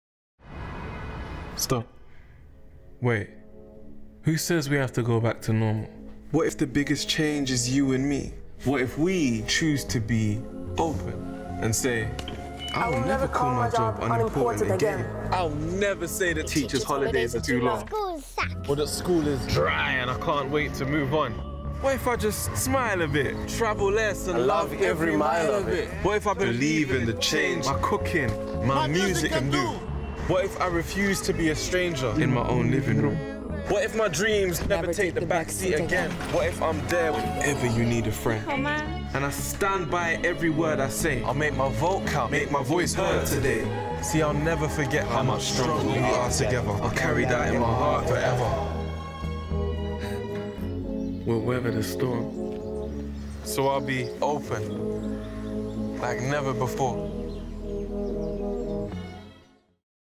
August 2020-COCA COLA ADVERT. OpenLikeNeverBefore. Coca-Cola & George the Poet present Open.
2020 Advert for Coca Cola.

Coca-Cola & George the Poet present: Open Like Never Before (60")

Wait stop, who says we have to go back to normal? This is our opportunity to start over - more humble, mindful, happier, braver, and better than we've ever been. The glass isn't half empty, it's half full and we can all make a difference when we are OpenLikeNeverBefore

We captured this sentiment in a beautiful cinematic poem performed by George the Poet, the game-changing voice of this generation.

All rights to this video lie with Coca Cola. Enjoy